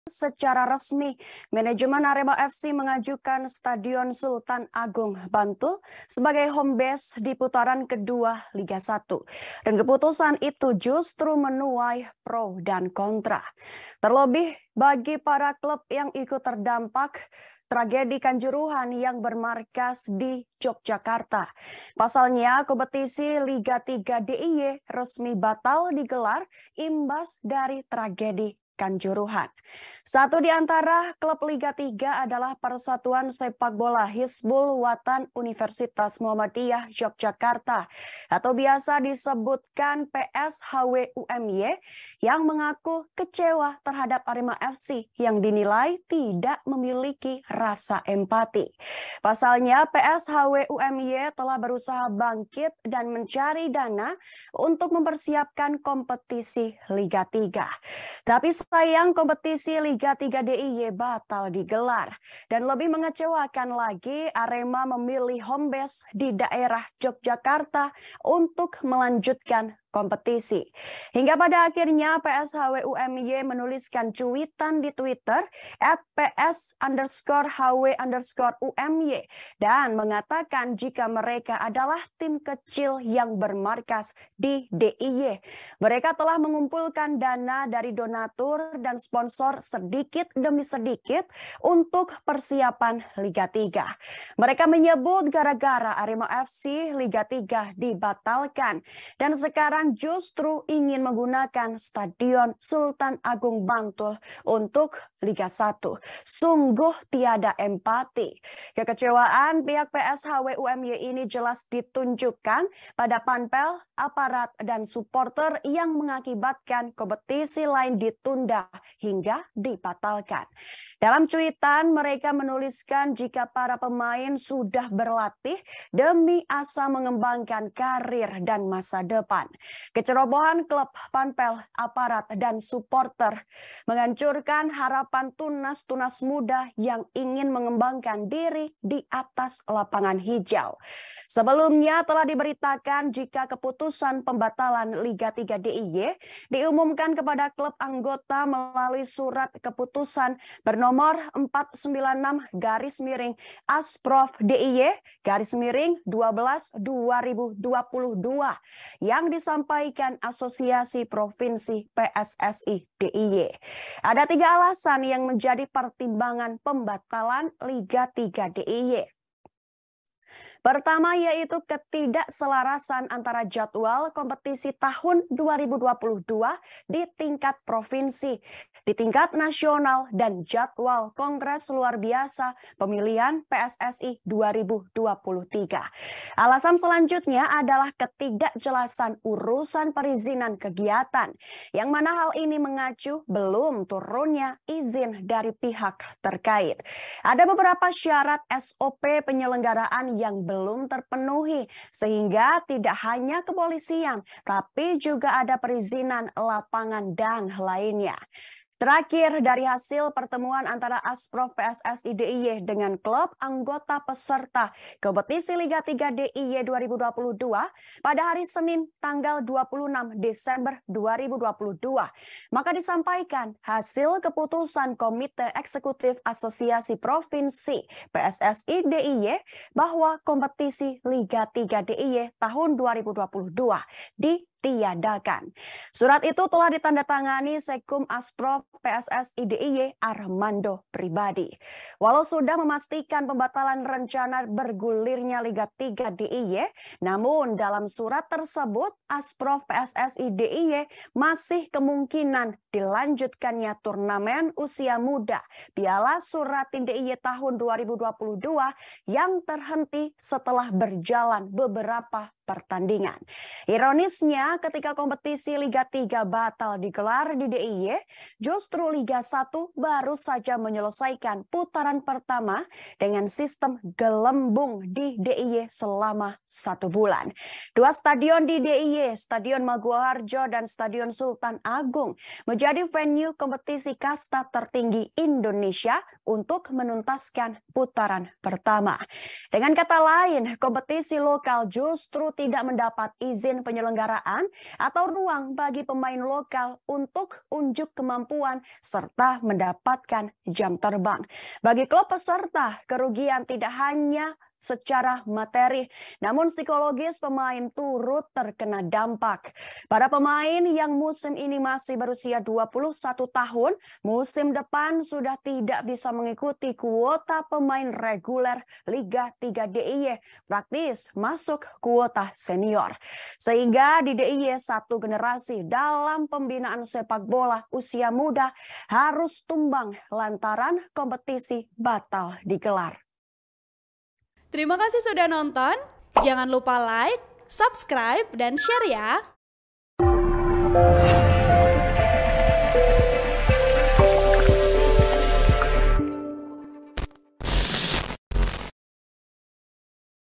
Klub Arema FC Resmi Ajukan Stadion Sultan Agung Bantul sebagai Homebase, Klub Liga 3 Lakukan Protes
TRIBUN-VIDEO.COM - Secara resmi, Manajemen Arema FC mengajukan Stadion Sultan Agung, Bantul sebagai homebase di putaran kedua Liga 1.

Dan keputusan itu, justru menuai pro dan kontra.

Terlebih bagi para klub yang ikut terdampak tragedi Kanjuruhan yang bermarkas di Yogyakarta.

Pasalnya kompetisi Liga 3 DIY resmi batal digelar imbas dari tragedi Kanjuruhan.

Satu di antara klub Liga 3 adalah Persatuan Sepakbola Hizbul Wathan Universitas Muhammadiyah Yogyakarta atau biasa disebutkan PS HW UMY, yang mengaku kecewa terhadap Arema FC yang dinilai tidak memiliki rasa empati.

Pasalnya, PS HW UMY telah berusaha bangkit dan mencari dana untuk mempersiapkan kompetisi Liga 3.

Tapi sayang, kompetisi Liga 3 DIY batal digelar.

Dan lebih mengecewakan lagi, Arema memilih homebase di daerah Yogyakarta untuk melanjutkan kompetisi.

Hingga pada akhirnya, PS HW UMY menuliskan cuitan di twitter @PS_HW_UMY dan mengatakan jika mereka adalah tim kecil yang bermarkas di DIY.

Mereka telah mengumpulkan dana dari donatur dan sponsor sedikit demi sedikit untuk persiapan Liga 3.

Mereka menyebut, gara-gara Arema FC Liga 3 dibatalkan, dan sekarang justru ingin menggunakan Stadion Sultan Agung (SSA) untuk Liga 1.

Sungguh tiada empati!

Kekecewaan pihak PS HW UMY ini jelas ditunjukkan pada panpel, aparat, dan suporter yang mengakibatkan kompetisi lain ditunda hingga dibatalkan.

Dalam cuitan, mereka menuliskan jika para pemain sudah berlatih demi asa mengembangkan karir dan masa depan.

Kecerobohan klub, panpel, aparat dan suporter kalian @AremafcOfficial menghancurkan harapan tunas-tunas muda yang ingin mengembangkan diri di atas lapangan hijau.

Sebelumnya telah diberitakan, jika keputusan pembatalan Liga 3 DIY diumumkan kepada klub anggota melalui surat keputusan (SK) bernomor 496/ASPROV-DIY/XII-2022 yang disampaikan Asosiasi Provinsi (Asprov) PSSI DIY.

Ada tiga alasan yang menjadi pertimbangan pembatalan Liga 3 DIY.

Pertama yaitu ketidakselarasan antara jadwal kompetisi tahun 2022 di tingkat Provinsi, di tingkat Nasional dan jadwal Kongres Luar Biasa (KLB) Pemilihan PSSI 2023.

Alasan selanjutnya adalah ketidakjelasan urusan perizinan kegiatan, yang mana hal ini mengacu belum turunnya izin dari pihak terkait

Ada beberapa syarat SOP penyelenggaraan yang belum terpenuhi, sehingga tidak hanya kepolisian, tapi juga ada perizinan lapangan dan lainnya.

Terakhir dari hasil pertemuan antara Asprov PSSI DIY dengan klub anggota peserta kompetisi Liga 3 DIY 2022, pada hari Senin tanggal 26 Desember 2022.

Maka disampaikan hasil keputusan Komite Eksekutif Asosiasi Provinsi PSSI DIY bahwa, Kompetisi Liga 3 DIY tahun 2022 ditiadakan.

Surat itu telah ditandatangani Sekum Asprov PSSI DIY, Armando Pribadi.

Walau sudah memastikan pembatalan rencana digulirkannya Liga 3 DIY, namun dalam surat tersebut Asprov PSSI DIY masih kemungkinan dilanjutkannya Turnamen Usia Muda Piala Soeratin DIY tahun 2022 yang terhenti setelah berjalan beberapa pertandingan.

Ironisnya ketika kompetisi Liga 3 batal digelar di DIY, justru Liga 1 baru saja menyelesaikan putaran pertama dengan sistem gelembung di DIY selama satu bulan.

Dua stadion di DIY, Stadion Maguwoharjo dan Stadion Sultan Agung menjadi venue kompetisi kasta tertinggi Indonesia untuk menuntaskan putaran pertama.

Dengan kata lain kompetisi lokal justru tidak mendapat izin penyelenggaraan atau ruang bagi pemain lokal untuk unjuk kemampuan serta mendapat jam terbang.

Bagi klub peserta, kerugian pasti tidak hanya secara materi, namun psikologis pemain turut terkena dampak.

Para pemain yang musim ini masih berusia 21 tahun, musim depan sudah tidak bisa mengikuti kuota pemain reguler Liga 3 DIY, praktis masuk kuota senior.

Sehingga di DIY satu generasi dalam pembinaan sepak bola usia muda harus tumbang lantaran kompetisi batal digelar.

(Tribun-Video.com)

Artikel ini telah tayang di Tribunnews.com dengan judul Arema FC Pilih Stadion Sultan Agung Jadi Homebase, Klub Liga 3 Protes: Sungguh Tiada Empati!,
Penulis: Siti Nurjannah Wulandari
Editor: bunga pradipta p